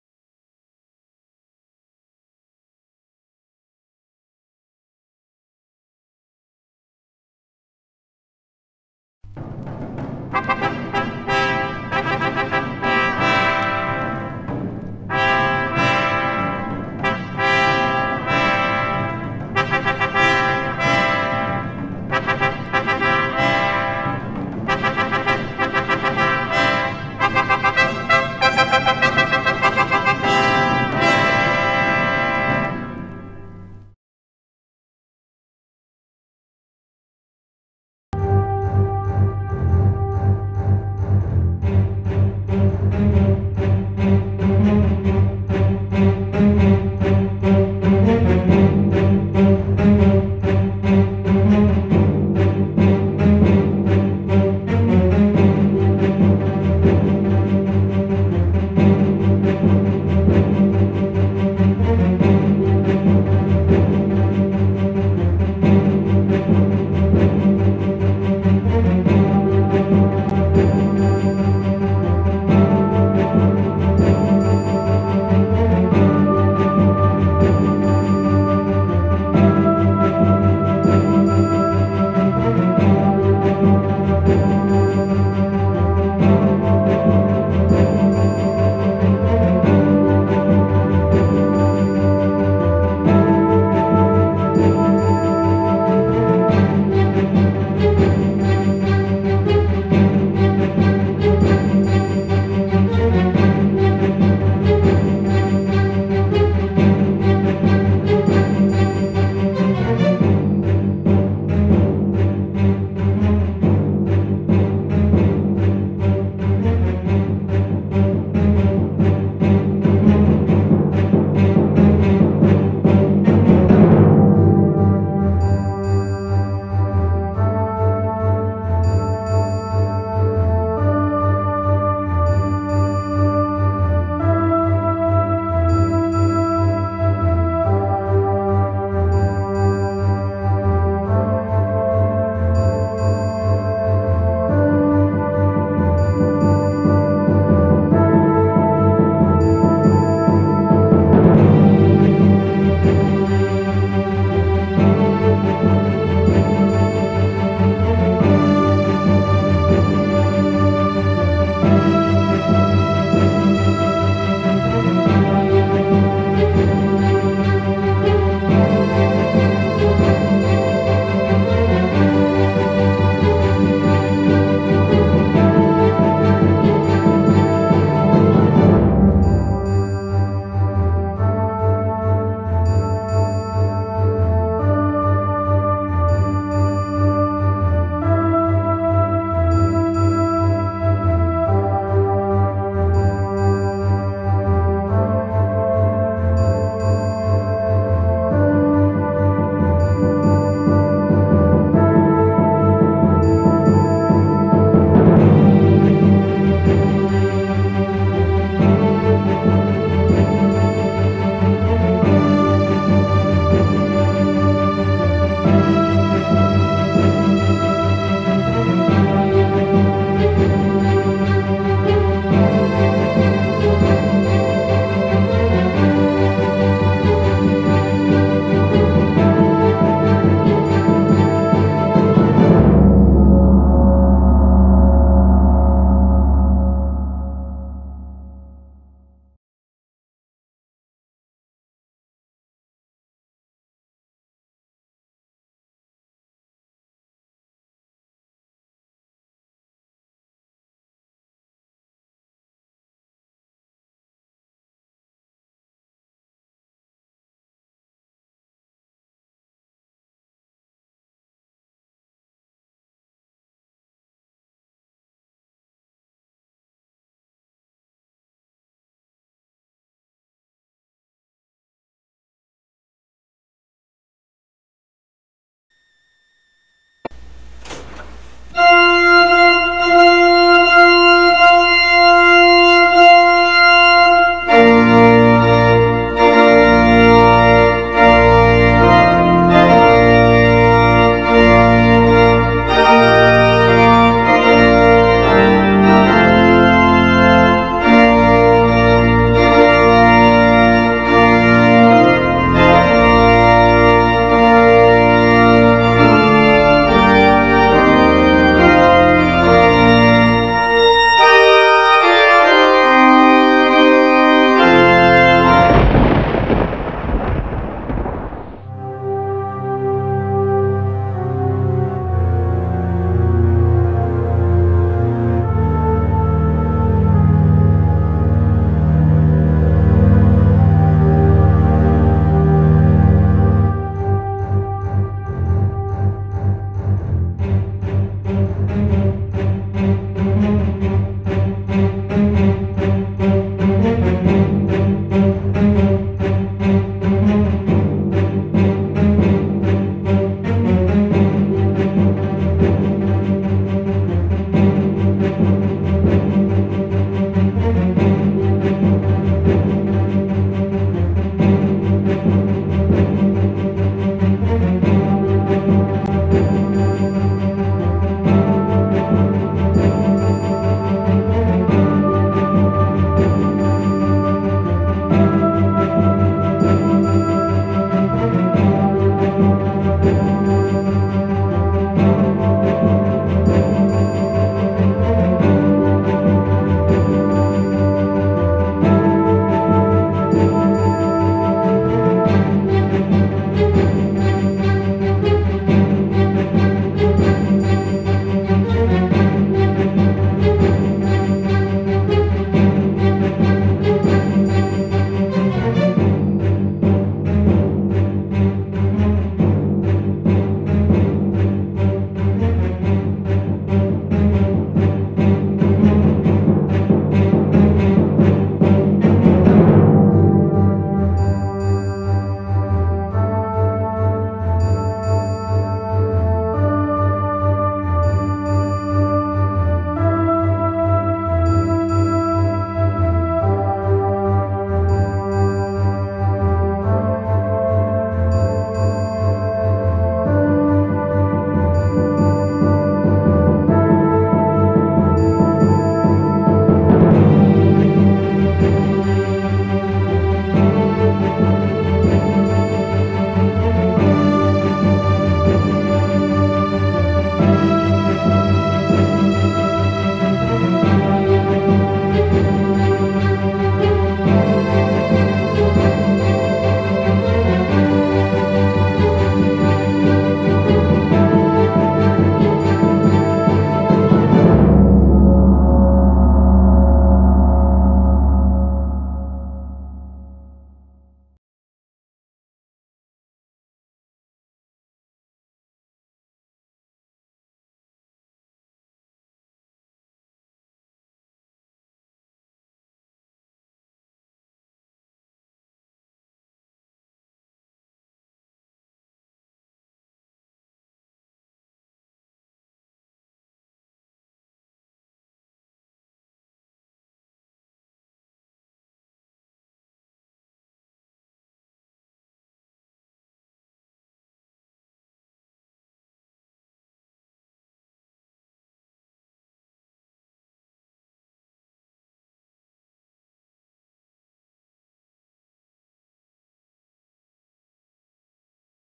BILL SHORTEN the reluctant hero in HD
Bill Shorten the Beaconsfield hero want's to be our next prime minister.What's he really like? Well here he is in his own words. Use full screen for readability